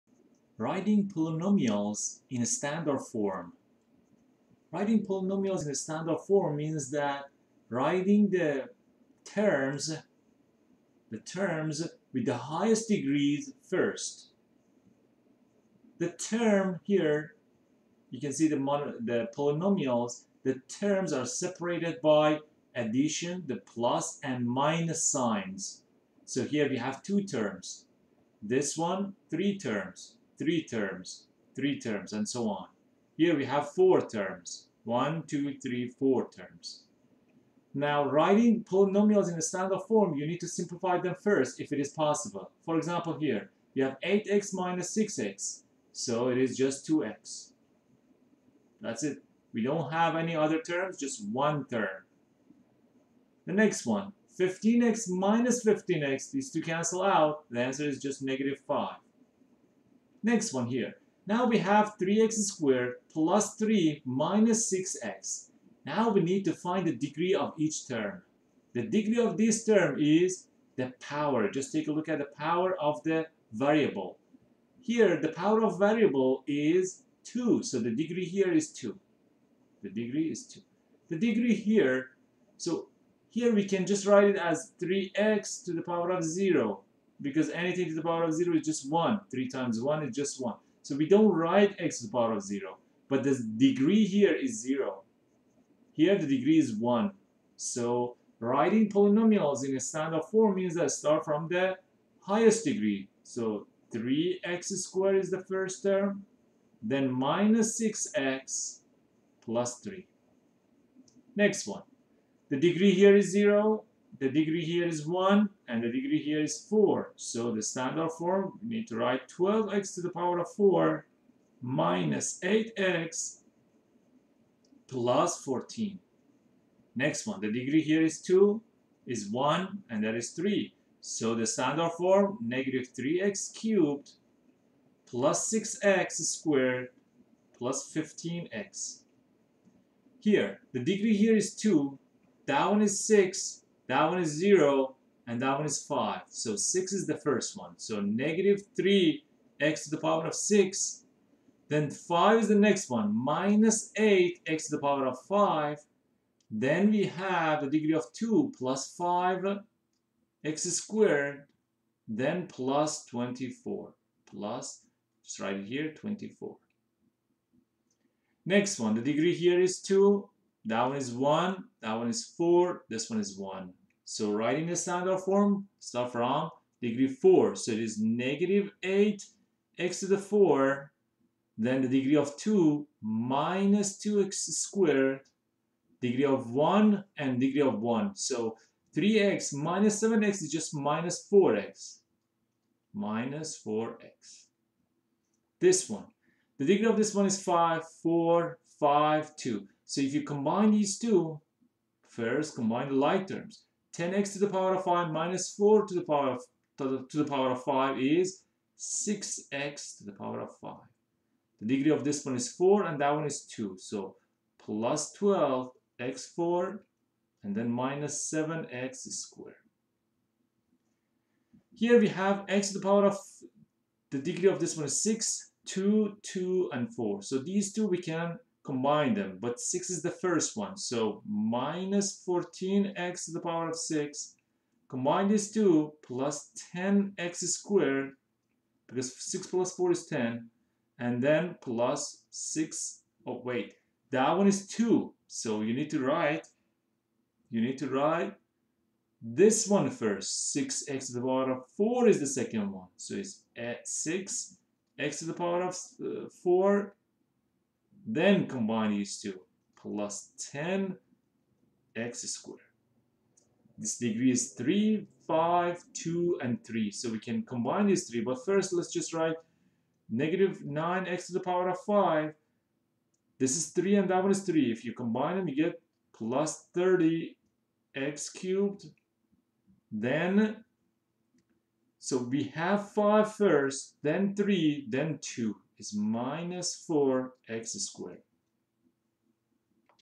Writing Polynomials in Standard Form
In this video, you learn how to write Writing Polynomials in Standard Form.

Published by Effortless Math Education
ww.EffortlessMath.com